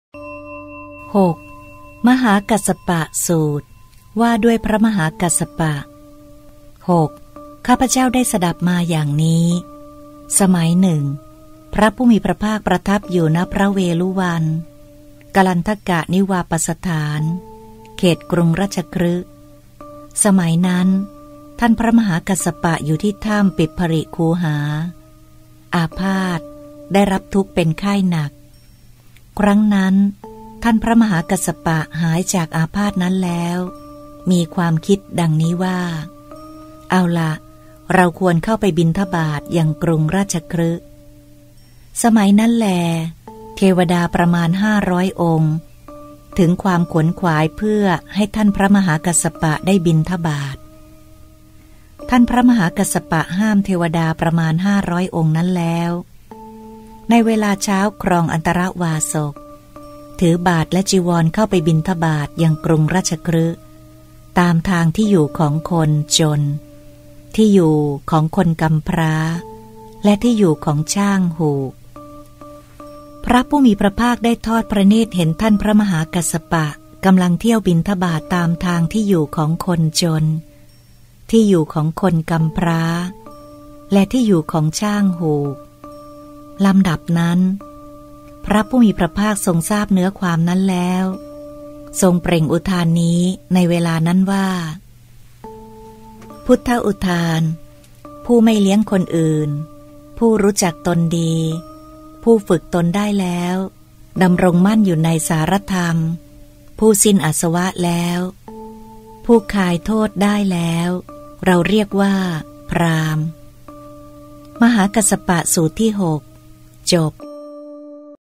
25-320 พระมหากัสสปะ
ว่าด้วยพระมหากัสสปะ
{๔๓} [๖] ข้าพเจ้าได้สดับมาอย่างนี้
สมัยหนึ่ง พระผู้มีพระภาคประทับอยู่ ณ พระเวฬุวัน กลันทกนิวาปสถาน A
เขตกรุงราชคฤห์ สมัยนั้น ท่านพระมหากัสสปะอยู่ที่ถ้ำปิปผลิคูหา อาพาธ ได้รับทุกข์
เป็นไข้หนัก ครั้งนั้น ท่านพระมหากัสสปะหายจากอาพาธนั้นแล้วมีความคิดดังนี้ว่า
“เอาละ เราควรเข้าไปบิณฑบาตยังกรุงราชคฤห์”
สมัยนั้นแล เทวดาประมาณ ๕๐๐ องค์ ถึงความขวนขวายเพื่อให้ท่านพระ
มหากัสสปะได้บิณฑบาต ท่านพระมหากัสสปะห้ามเทวดาประมาณ ๕๐๐ องค์นั้นแล้ว
ในเวลาเช้าครองอันตรวาสก ถือบาตรและจีวรเข้าไปบิณฑบาตยังกรุงราชคฤห์
ตามทางที่อยู่ของคนจน ที่อยู่ของคนกำพร้า และที่อยู่ของช่างหูก
พระผู้มีพระภาคได้ทอดพระเนตรเห็นท่านพระมหากัสสปะ กำลังเที่ยวบิณฑบาต
ลำดับนั้น พระผู้มีพระภาคทรงทราบเนื้อความนั้นแล้ว จึงทรงเปล่งอุทานนี้
ในเวลานั้นว่า
พุทธอุทาน B
ผู้ไม่เลี้ยงคนอื่น ผู้รู้จักตนดี
ผู้ฝึกฝนตนได้แล้ว ดำรงมั่นอยู่ในสารธรรม C
ผู้สิ้นอาสวะแล้ว ผู้คายโทษได้แล้ว D
เราเรียกว่า พราหมณ์
มหากัสสปสูตรที่ ๖ จบ
เชิงอรรถ
A หมายถึงสถานที่สำหรับพระราชทานเหยื่อแก่กระแต
(ม.มู.อ. ๒/๒๕๒/๔๑-๔๒, ขุ.อุ.อ. ๖/๖๒)
B พุทธอุทานนี้ ทรงเปล่งแสดงความเป็นผู้มั่นคงของพระขีณาสพ
โดยทรงมุ่งแสดงความเป็นผู้มักน้อยอย่างยิ่ง เป็นหลัก (ขุ.อุ.อ. ๖/๖๔)
C สารธรรม หมายถึงวิมุตติสาระ (ขุ.อุ.อ. ๖/๖๖)
D คายโทษ หมายถึงคายโทษมีราคะเป็นต้น (ขุ.อุ.อ. ๖/๖๖)